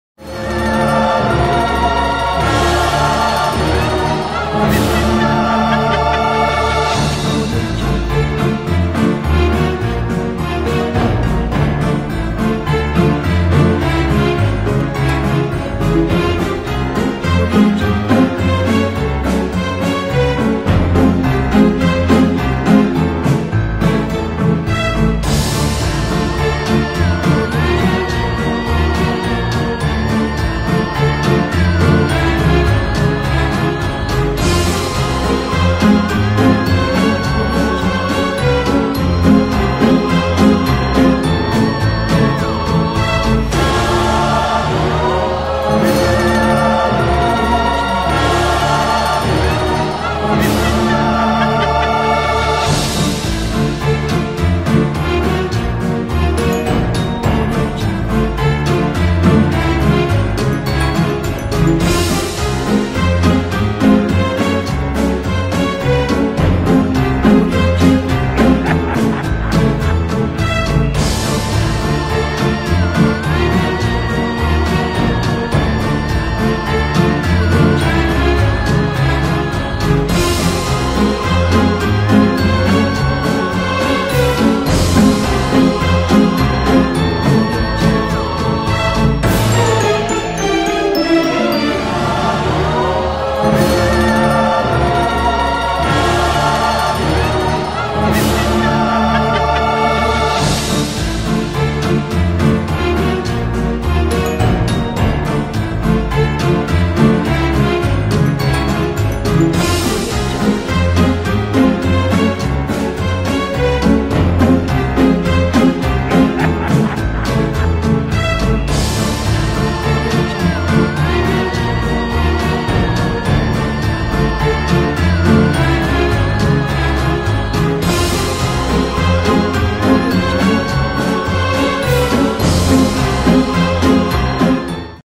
Halloween | After Effects Project Files - Videohive template
Halloween Titles is a 3D Element V 2 After Effects template 3 unique 3D title animations with the 6 Different Color Animated Background & Each Logo 6 different Color Combination All text and date are editable Perfect for all kinds of Halloween party invitations, intros, openers, titles Super easy to customized and editable colors & Text Detailed Tutorial Video File include in Project Fonts Download Music is not included,